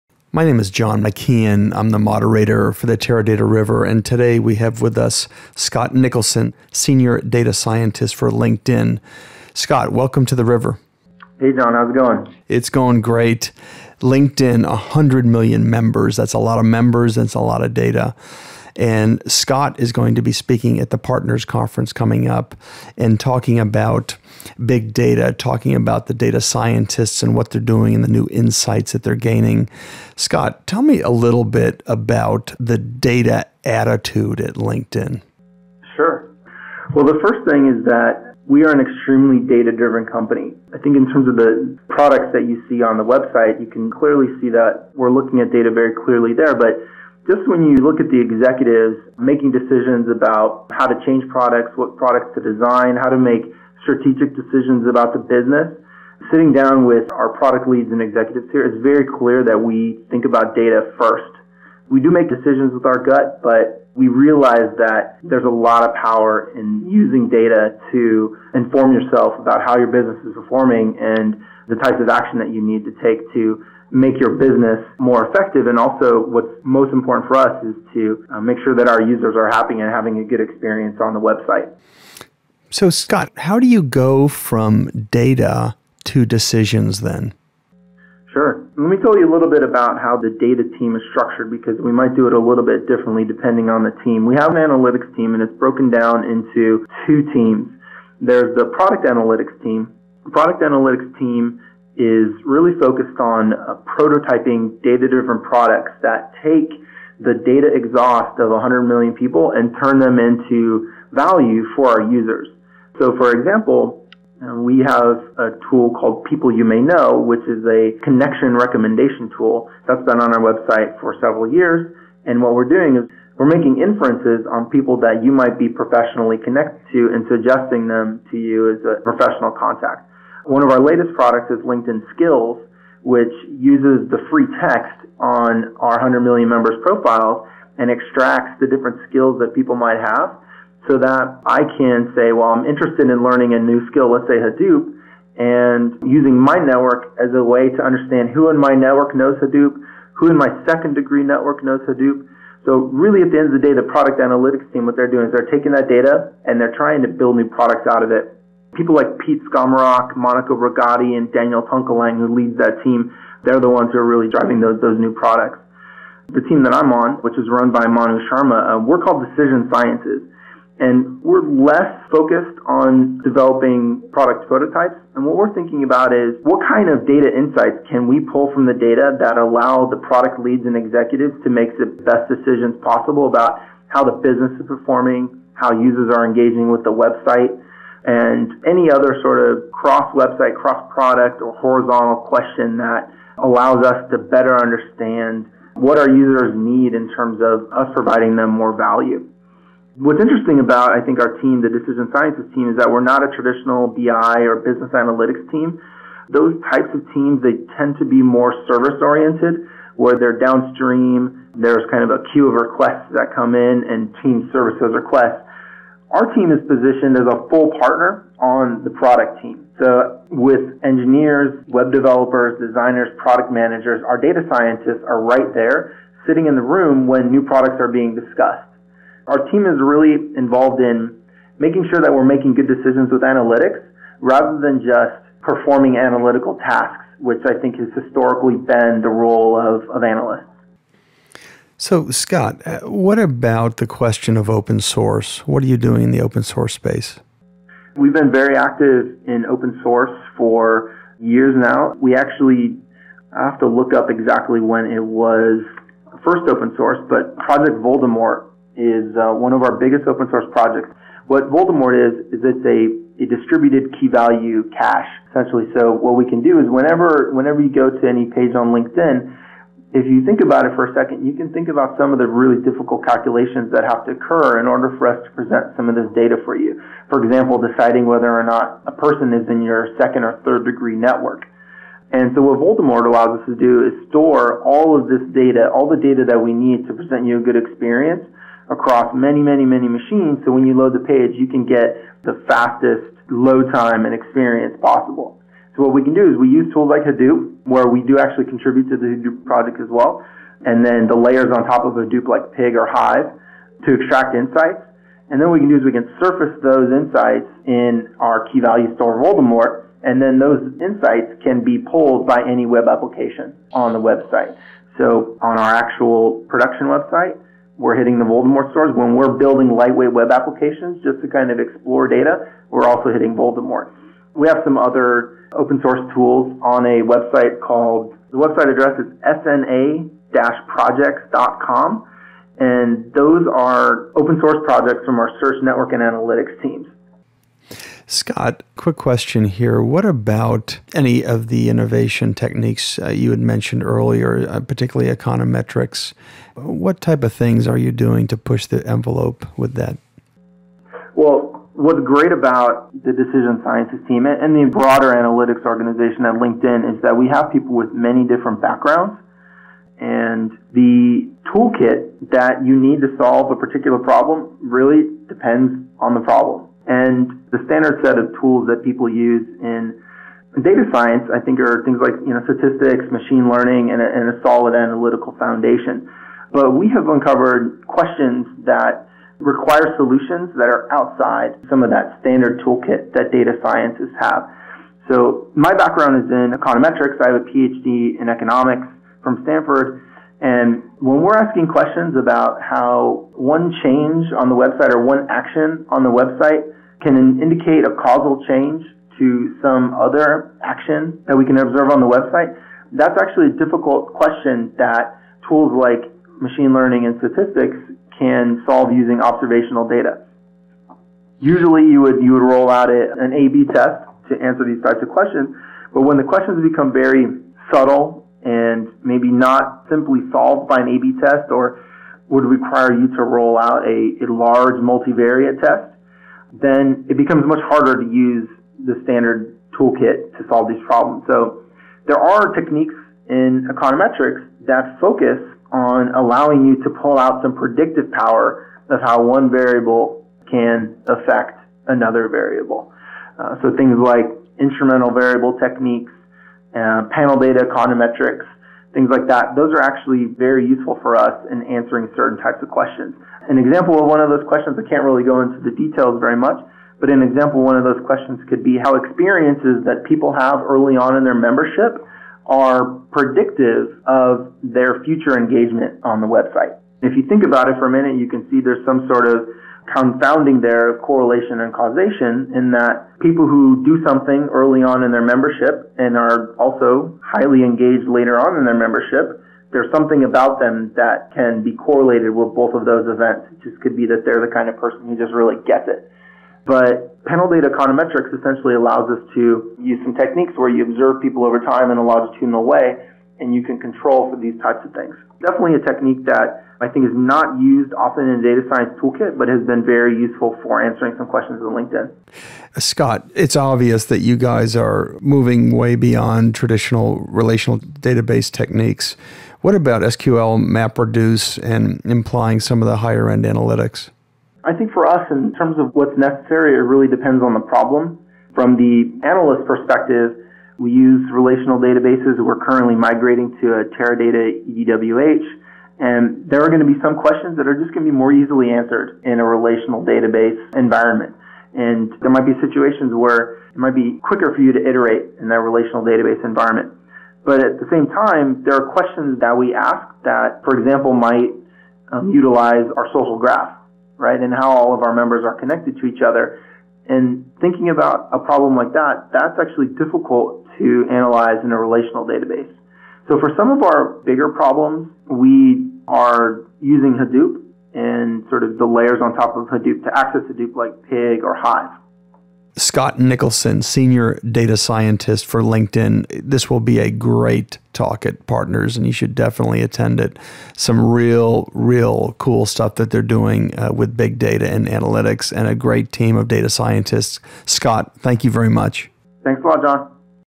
Teradata PARTNERS 2011 Sneak Preview: Linkedin - Scott Nicholson
Scott Nicholson
Senior Data Scientist

PARTNERS Session:
Sunday, October 2 @ 4:00PM
Ballroom 20A

• The "Data Attitude" at LinkedIn
• Decisions come using data FIRST
• Open Source data at LinkedIn
• Pushing the envelope with "Econometrics"
• Using diverse techniques to pull data that is otherwise too subtle for traditional techniques
• Advantages of having a team with diversified skills
• Moving beyond traditional relational database techniques

"Companies who compete on analytics and delivering data-driven services need to iterate quickly on big data. This enables rapid data exploration to identify unknown relationships and trends to create new products and services. Come to this session to see how LinkedIn has created a core competency around analytics. Understand about the techniques and technologies LinkedIn data scientists use, including the Aster Data analytic platform, to create data-driven products such as People You May Know. See real-life examples of how Aster Data's SQL-MapReduce enables analytics that can't be done (easily) in traditional relational database systems. Get ideas of how to apply iterative big data analytics in your own organization and enable your own analytics center of innovation."